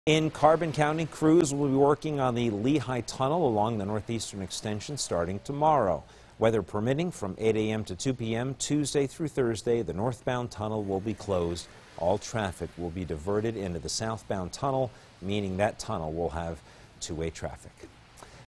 Lehigh Tunnel Traffic Alert
Crews will start work on the Northbound side of the Lehigh Tunnel starting tomorrow.